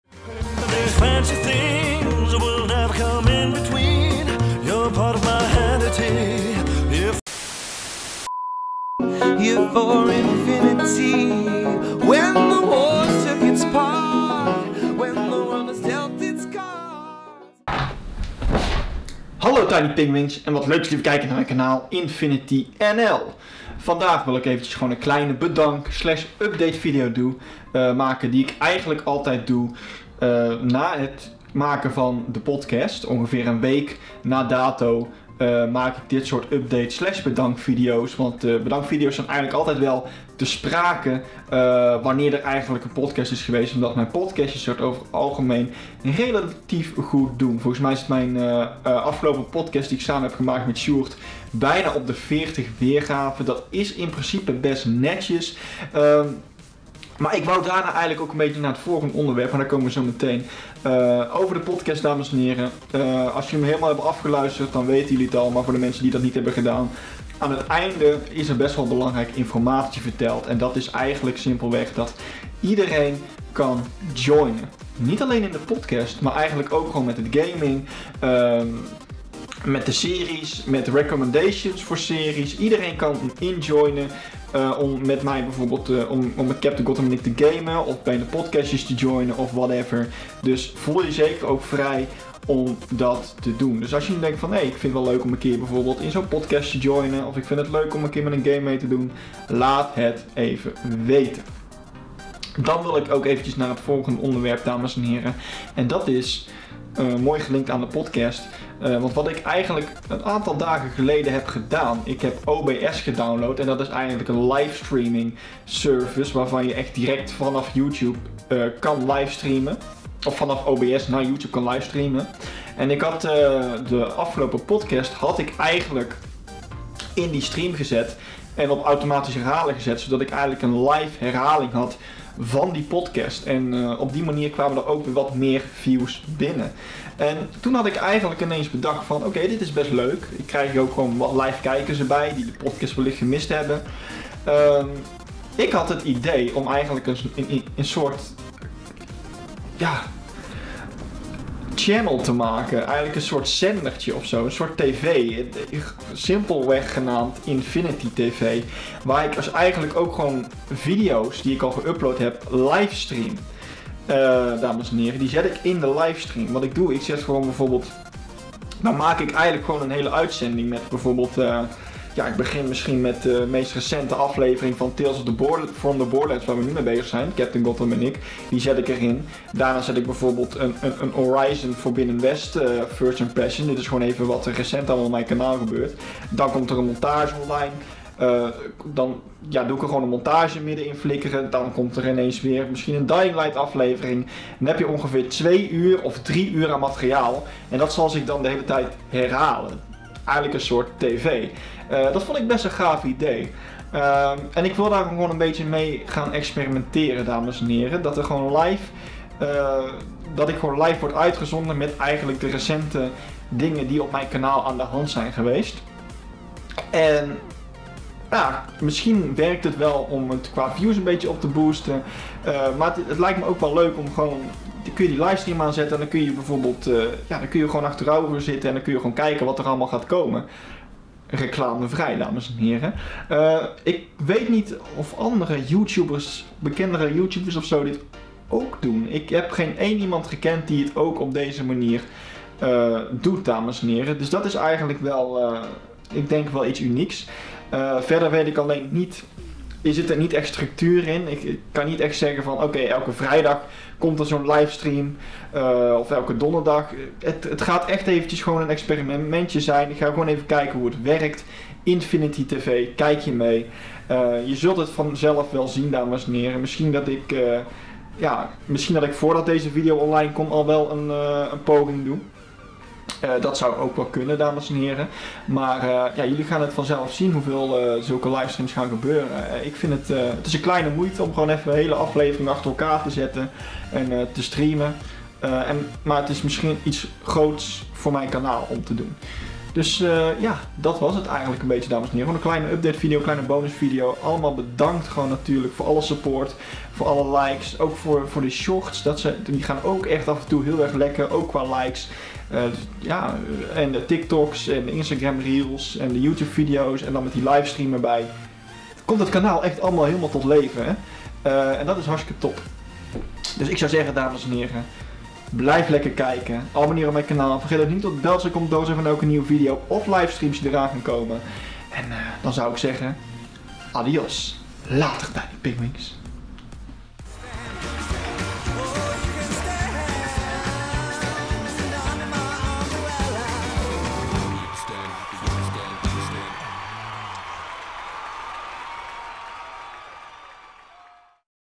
Infinity tv! 🐧
Hallo tiny pinguïns!  Leuk dat jullie weer kijken naar een bedank/updatevideo waarin ik alles bespreek over mijn kanaal!  In deze video wil ik jullie weer bedanken voor de goede support op de afgelopen paar video’s!  En dan met name de podcast die ik samen met sjoerd heb gerealiseerd!   Ook ga ik in deze video mijn nieuwe concept uitleggen genaamd Infinity tv!  Dus?  Kijk je mee?

Met medewerking van…